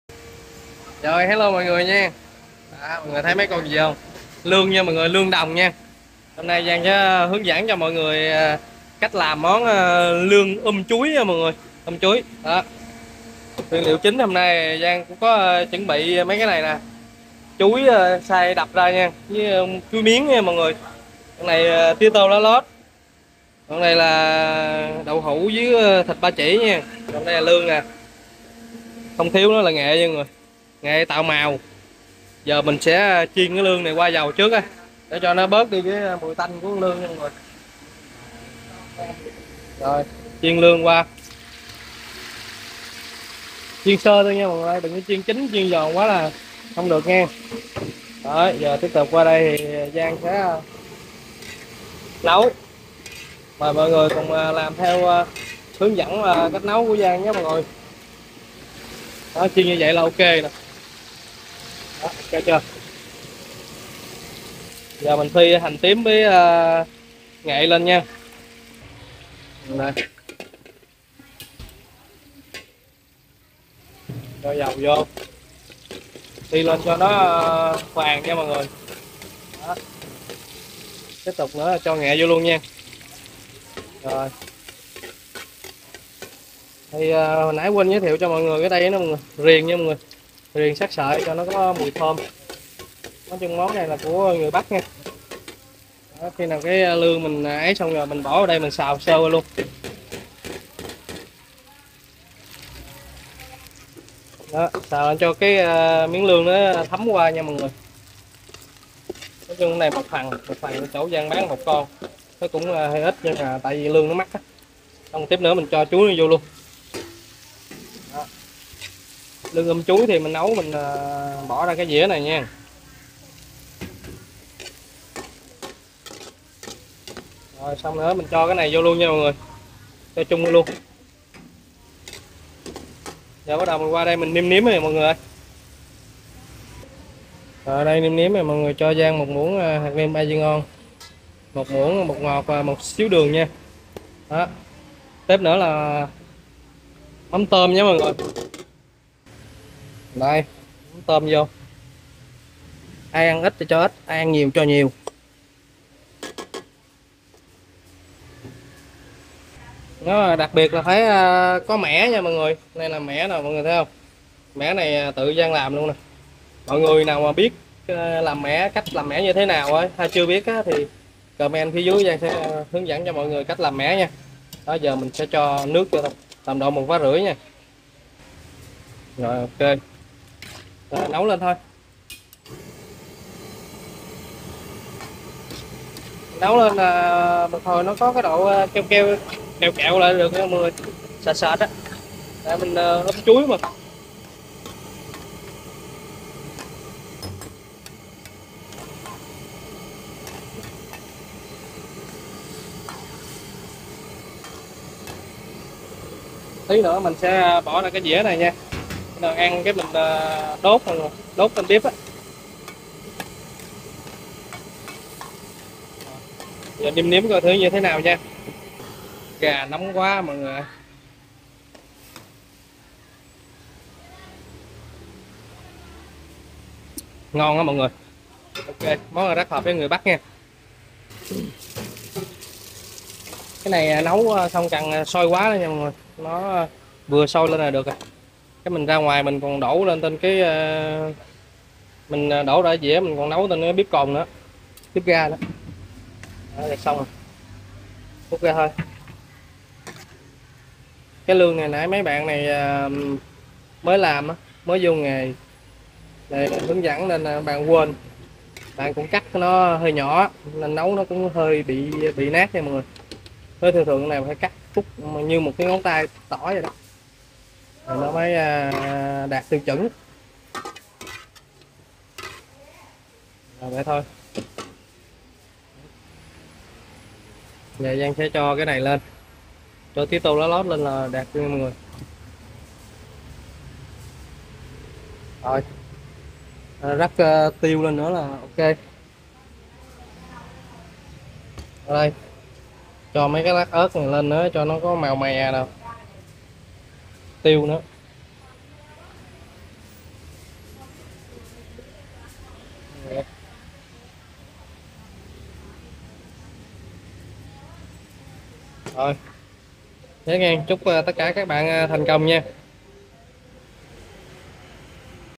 Cách nấu món Lươn đồng um chuối đậu ,miền Bắc, Nam gì cũng ăn được hết
Chào mừng mọi người đã đến với video của Giang, chúc mọi người  sức khỏe và thành công.